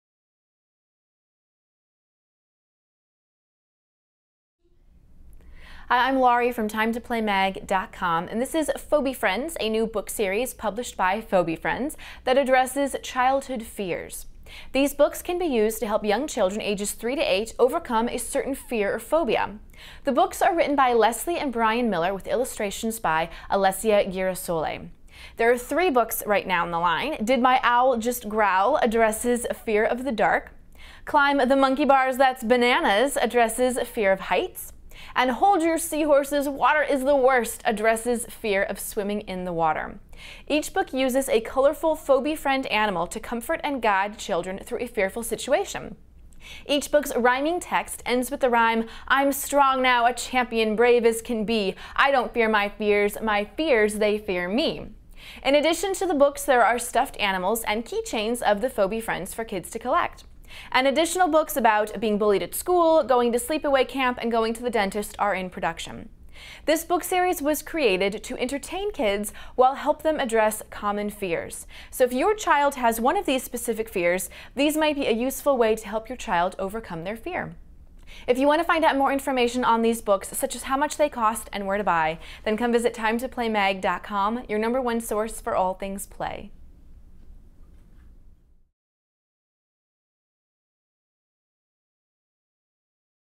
Fobie Friends Books published by Fobie Friends
For the full review, where it's in stock, and how much it costs,

Fobie Friends is a new book series that addresses childhood fears in an entertaining and informative way. These books can be used by teachers, counselors, educators, and parents to help young children overcome a certain fear or phobia. The books are written by Leslie and Brian Miller with illustrations by Alessia Girasole.

There are three books in the series. Did My Owl Just Growl? addresses the fear of the dark. Climb the Monkey Bars? That's Bananas! addresses the fear of heights. Hold Your Seahorses! Water is the Worst addresses the fear of swimming in water. The characters in each book (a blue owl, a purple gorilla, and a seahorse) depict a more child-friendly version of each phobia. Each book uses a colorful and engaging Fobie Friend animal to comfort and guide a child through a fearful situation. Each book's rhyming text ends with the rhyme, "I'm strong now, a champion, brave as can be. I don't fear my fears, my fears they fear me!"